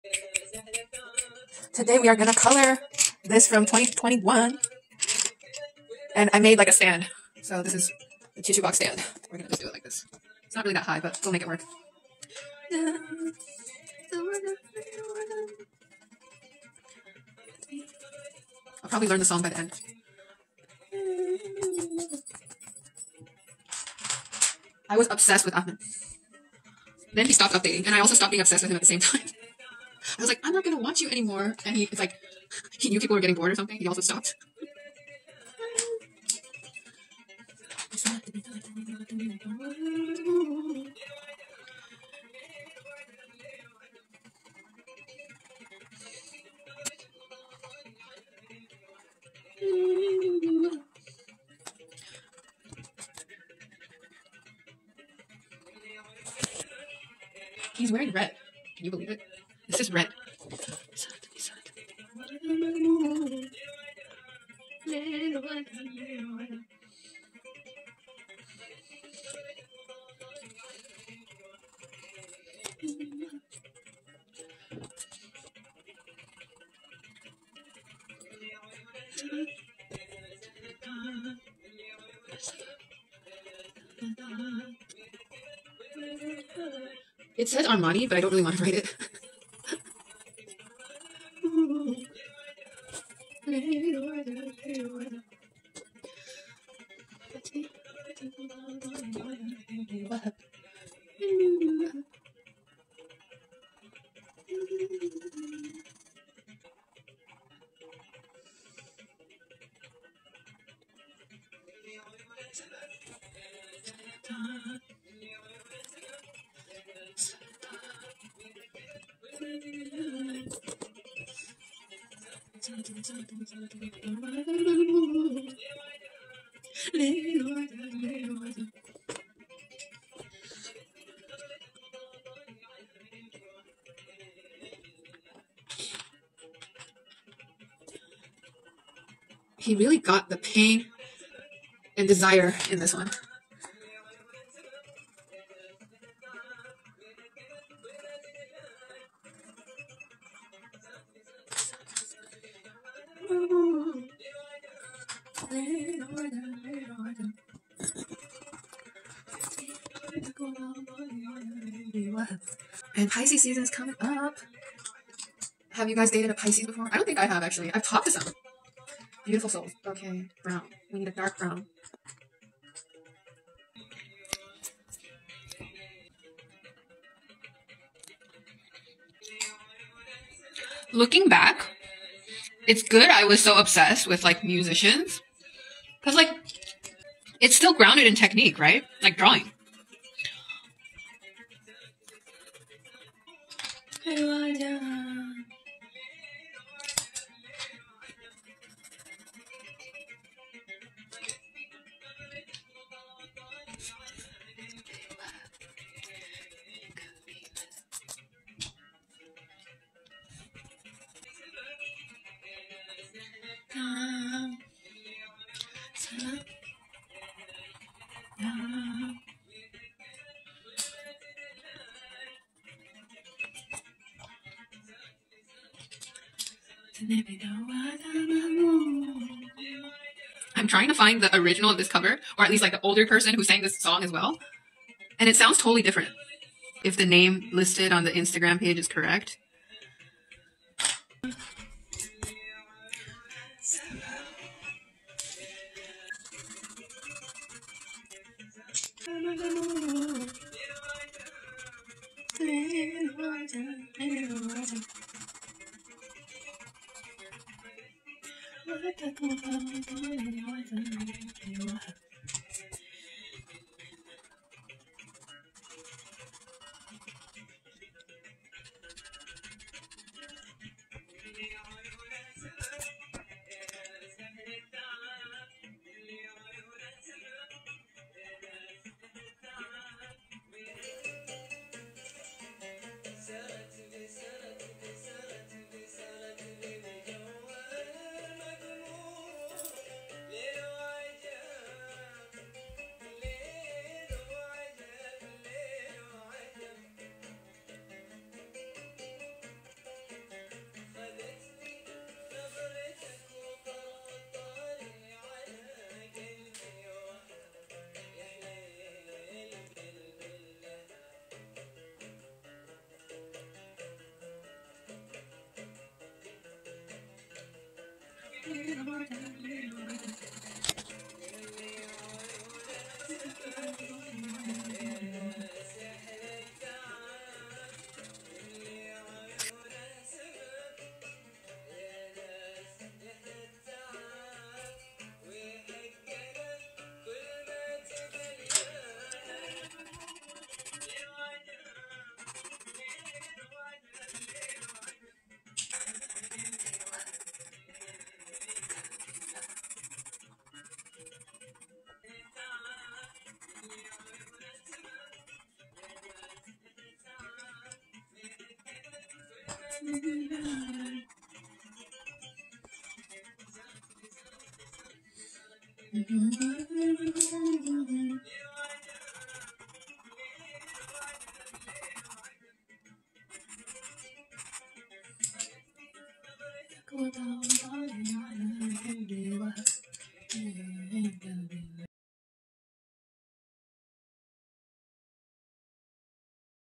pencil coloring 🎨 of Ahmed Bashiri the oud player
original song by عمر_كدرس